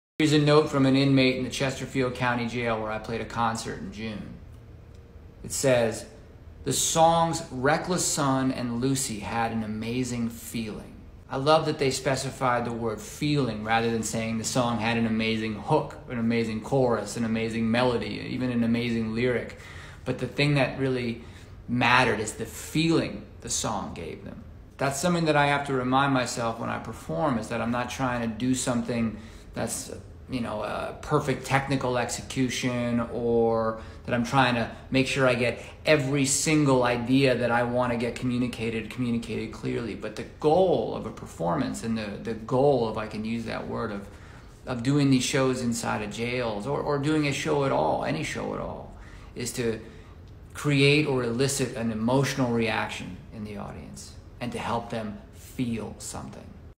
This card from an inmate at HARP reminded me what’s really important about a song and a performance
At the end of the day, all that matters is if the show made someone feel something.